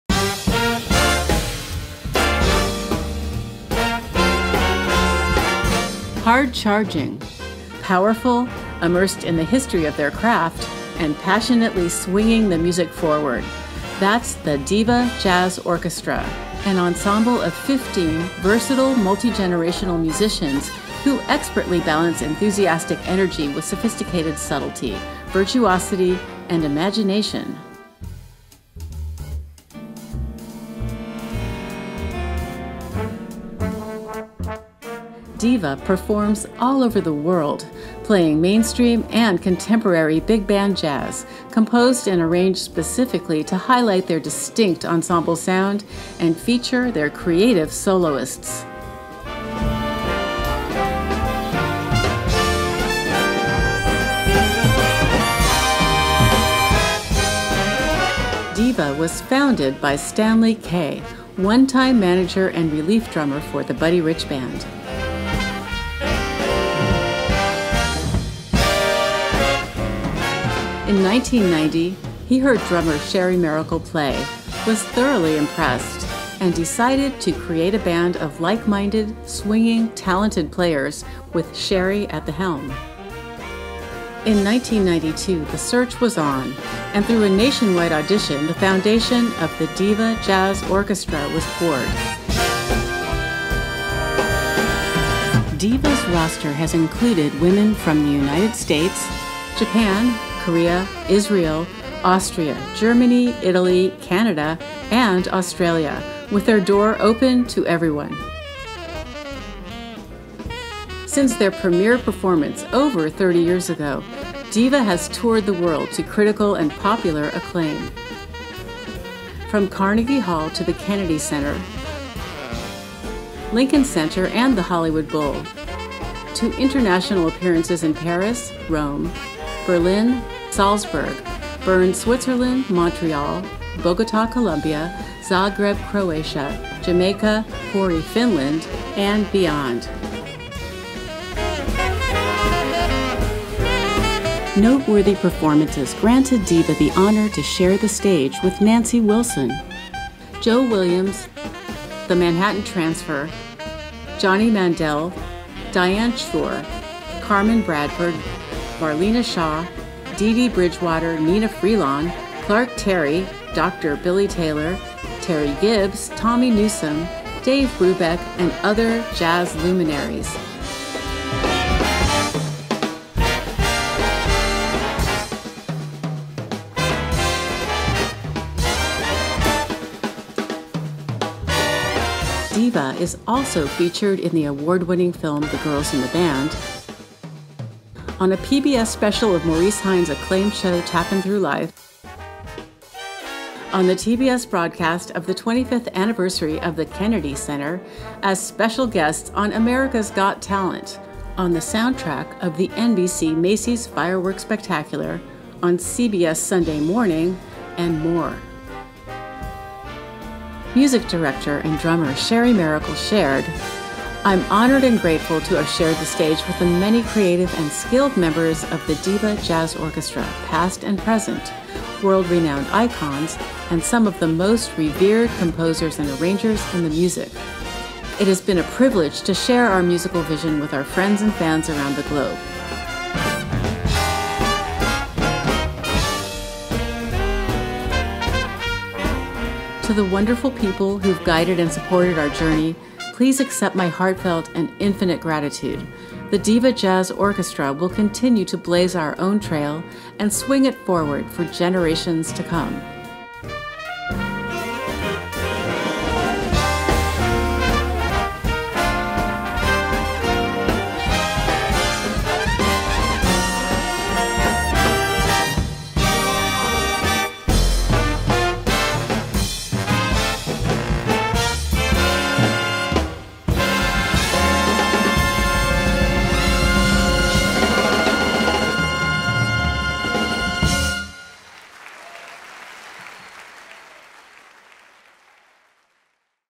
The DIVA Jazz Orchestra is "30!"
I've been honored to be the leader and drummer for The DIVA Jazz Orchestra more than half of my life.